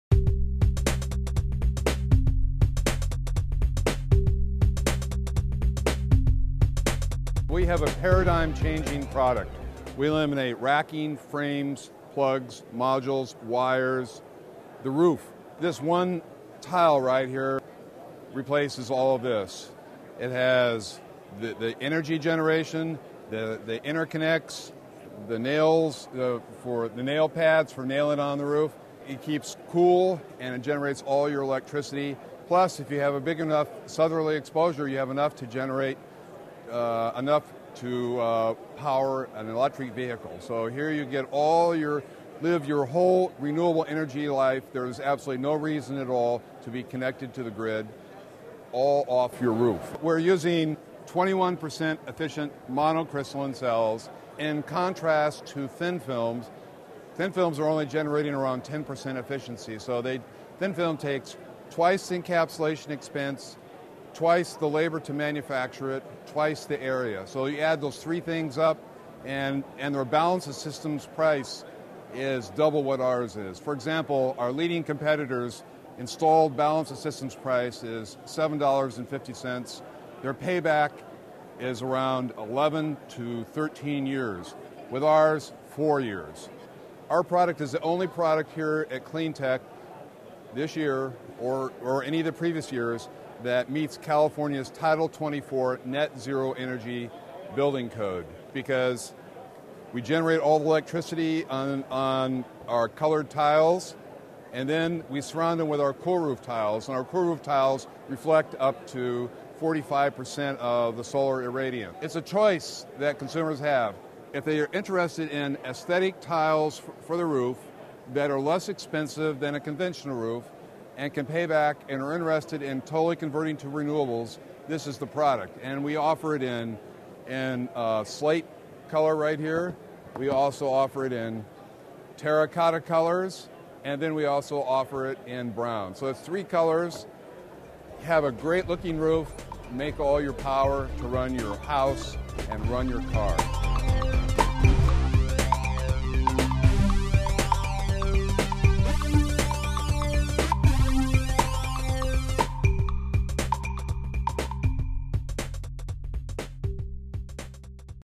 Redwood Renewables solar tiles
Redwood Renewables develops and manufactures integrated solar roofing tiles designed to accommodate both crystaline and thin film technology for residential buildings.

In this interview, the company's CEO Tom Faust explains their products' competitive advantages, aesthetic appeal and how it's possible for their customers to &quot;live an entire renewable-energy life, without connecting to the grid, all from the roof&quot;.

Redwood Renewables distribute their products through residential roofing companies.